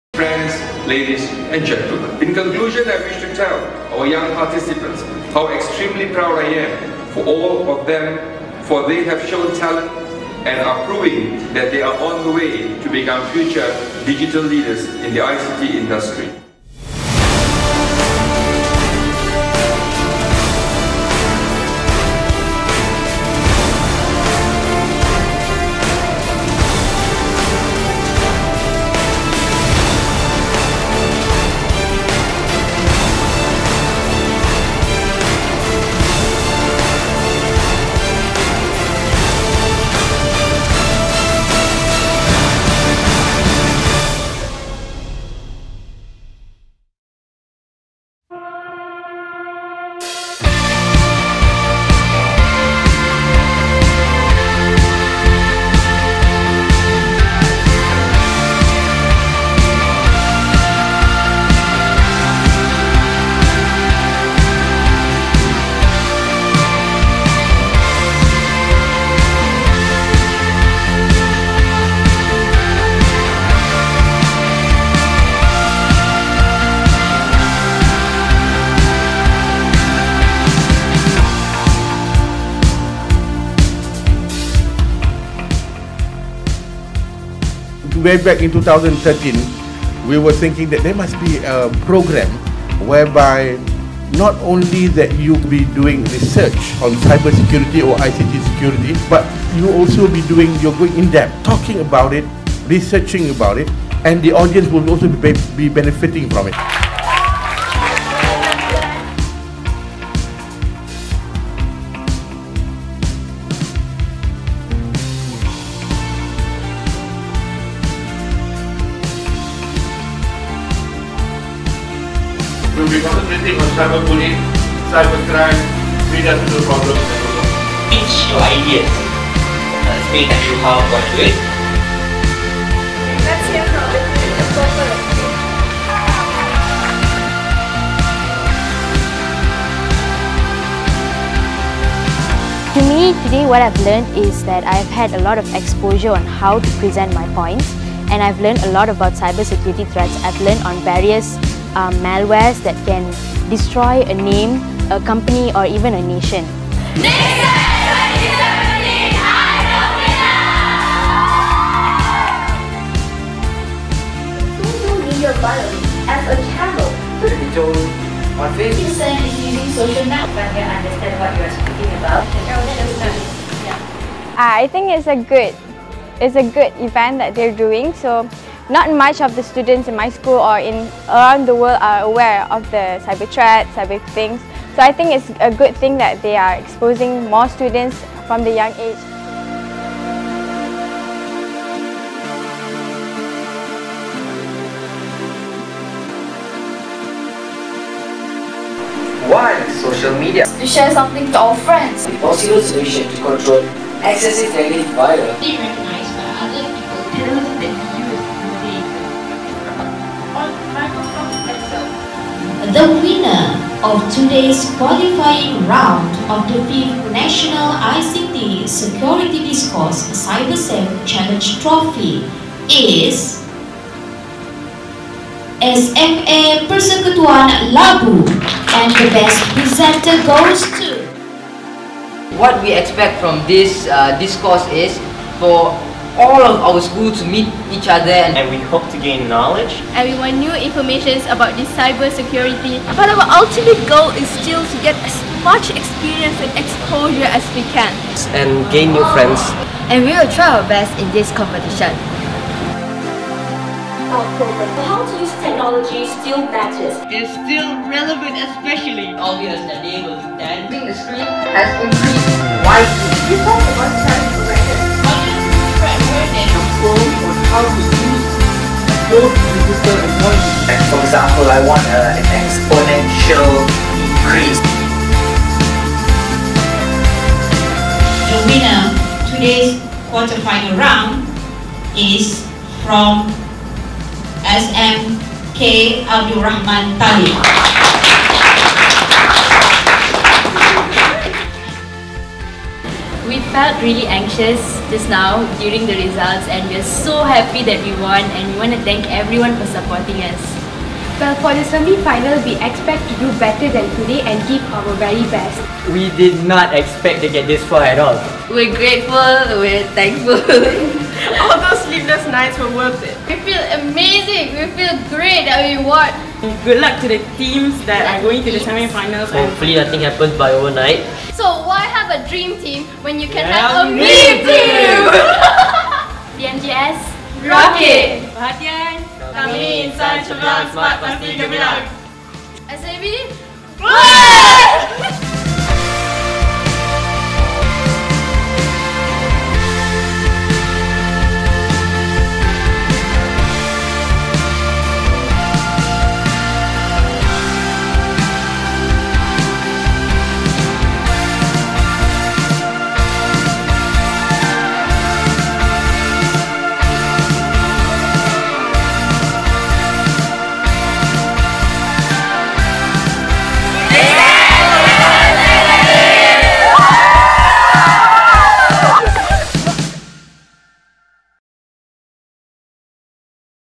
CyberSAFE In Schools: NICTSeD CyberSAFE Challenge Trophy 2017: Preliminary Rounds , BTPN Kedah
National ICT Security Discourse (NICTSeD) is Malaysia's first national ICT security school discourse with Support from Educational Technology Division (BTP), Ministry of Education (MOE) Malaysia is part of a series of activities under CyberSAFE™, a program to raise awareness on the importance of cyber safety and personal information security in Malaysia.

The 2017 Preliminary Round was held at BTPN Kedah. An elite group of 16 selected schools representing each state competing to showcase themselves discoursing to reach the grand final in Kuala Lumpur.